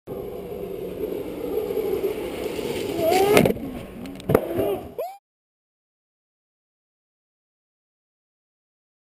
Jan's jump
Jan Tarradas jumped with his Street luge more than 12 meters...